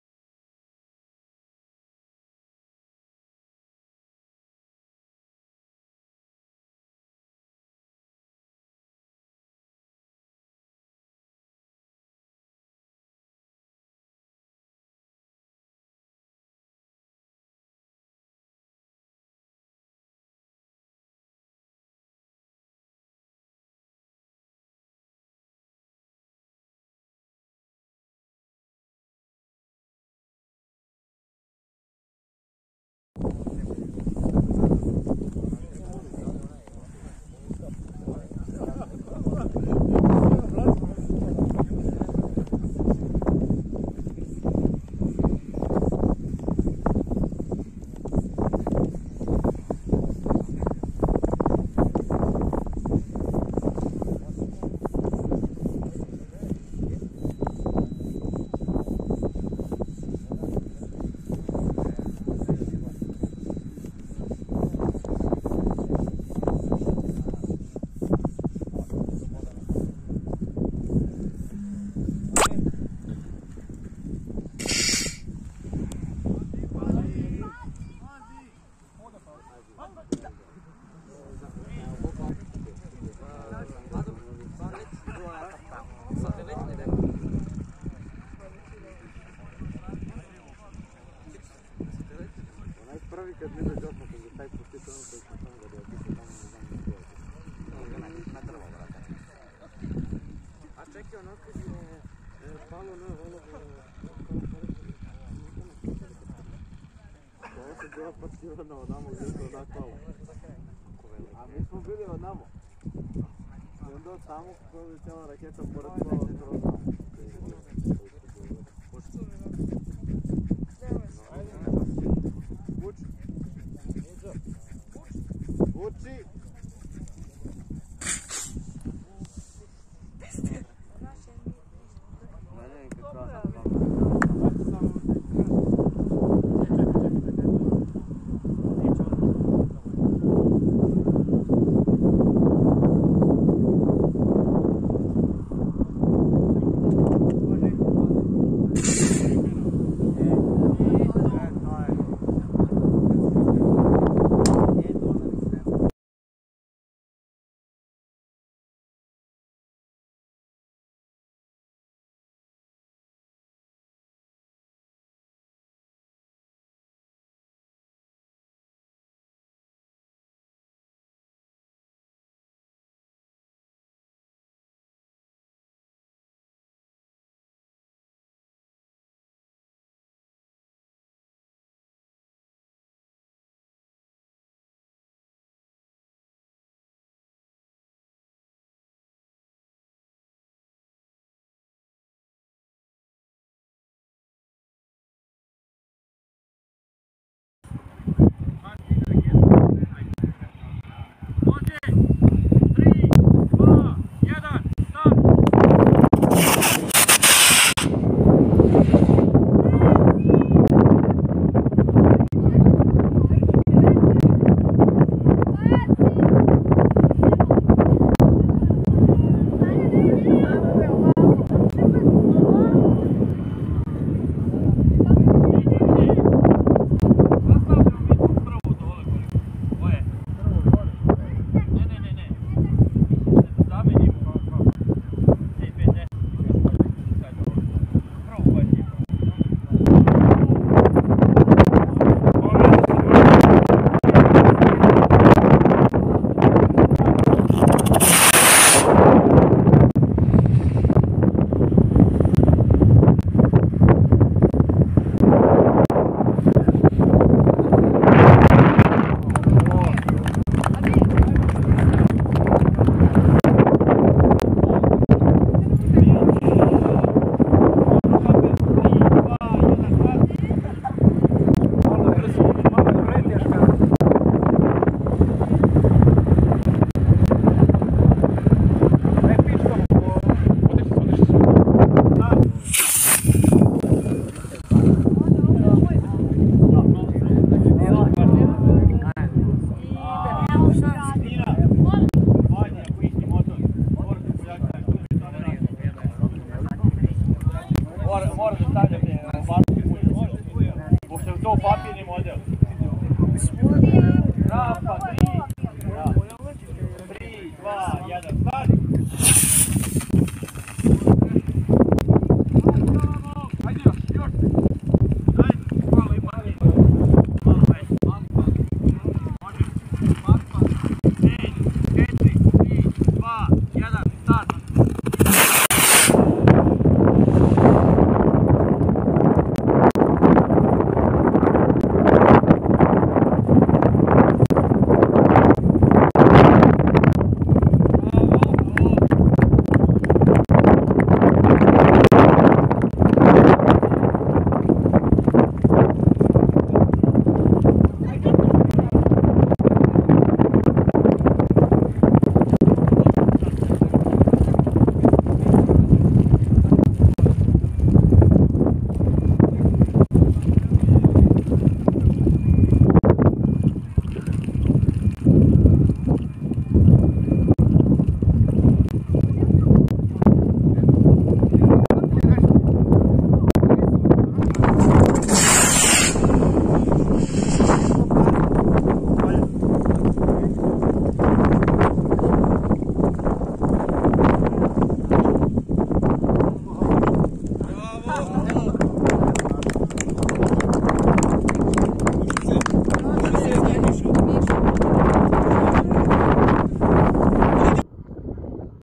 Serbian CanSat/Rocketry Competition - National competition as Phase 1 of the WCRC
9th April at Airport Veliki Radinci, Vojvodina, Serbia

Categories:
Primary school level
Secondary/High school level
University level

Water Rockets without CanSats;
Solid motor Rockets with CanSats;

Two CanSats in one Rocket as payload.